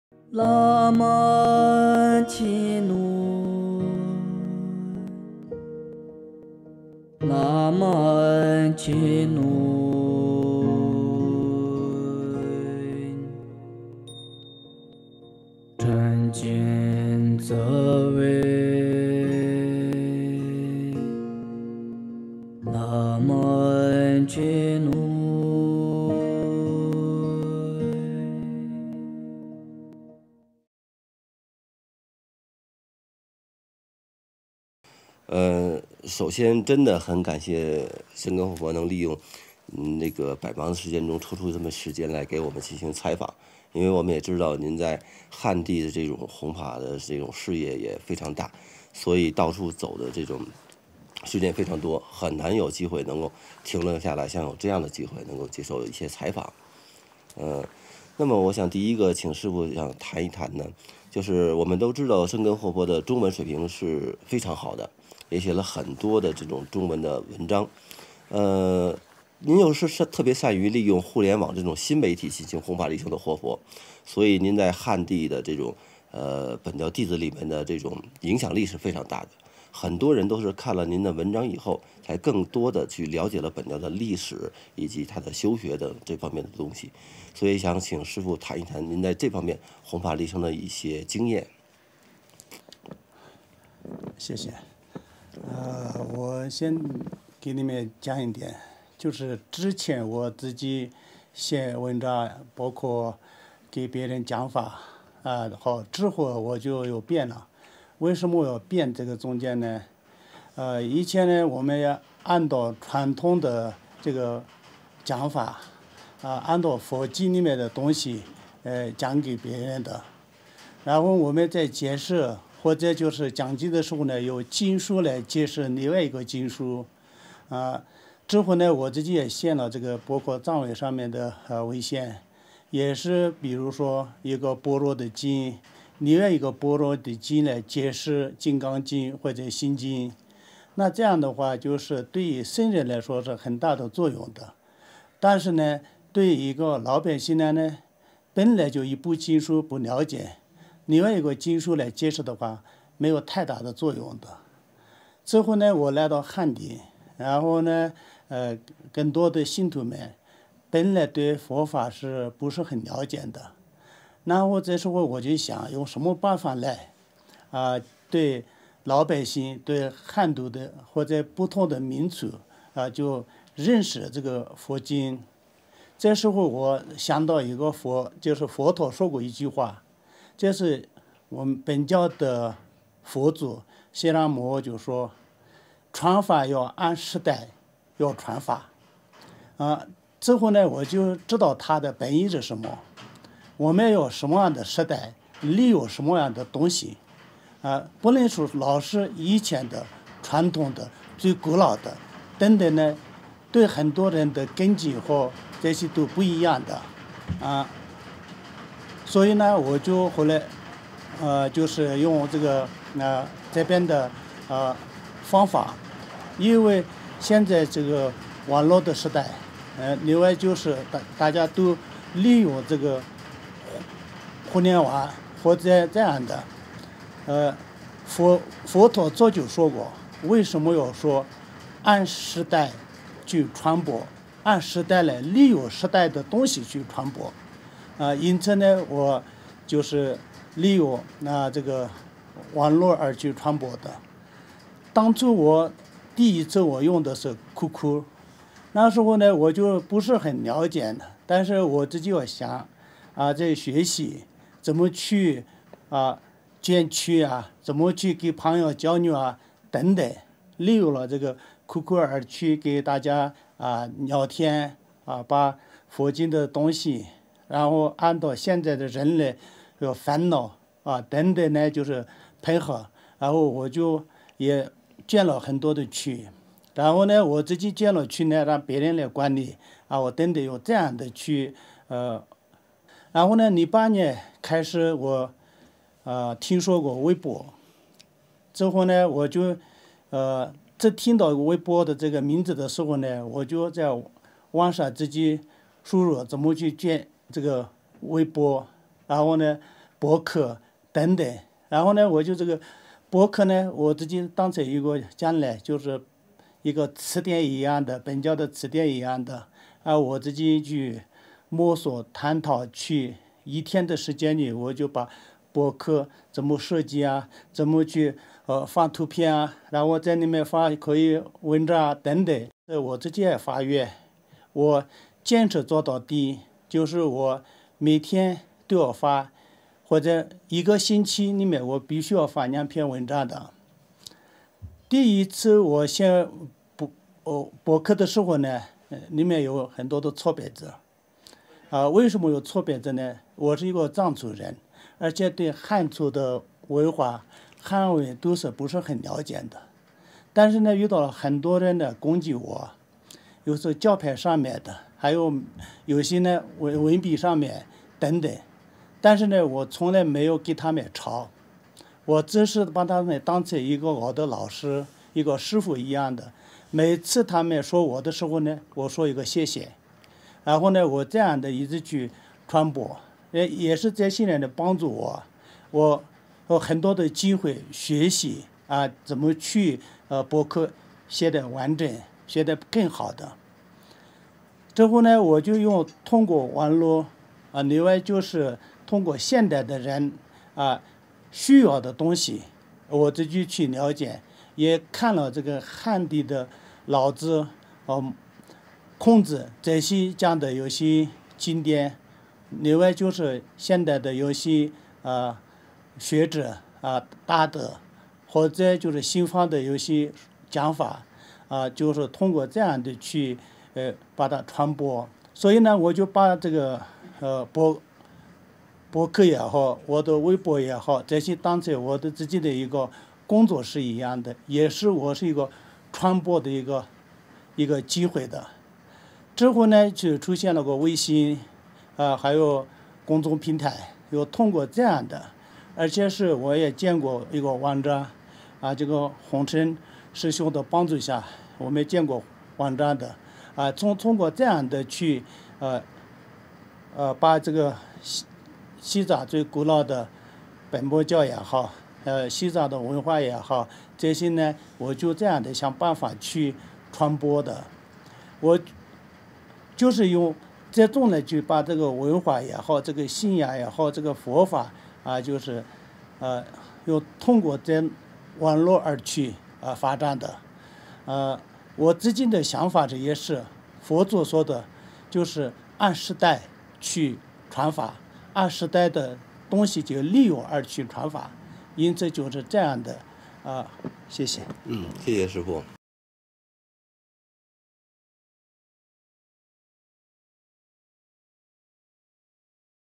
The interview Tulku Senghe, Experience of Modern Network in the Dissemination of Dharma 现代化网络弘法的经验
The interview of Tulku Senghe, Experience of Modern Network in the Dissemination of Dharma  生根活佛 现代化网络弘法的经验

The interview series of Tulku Senghe a is jointly recorded by Yungdrung Culture Studio and Beijing Yi Tai Vision Film Industry.

Tulku Senghe: at the age of 2, he was recognized as the reincarnated Tulku of Jetdrel Senghe Wangtsön of the Nyingma School, and studied the teaching methods of the Nyingma School and the Sakya School. Later, he went to Aba Nangzhig Monastery to study and chose to complete his career of promoting Dharma and benefiting sentient beings as the Tulku of Yungdrung Bon.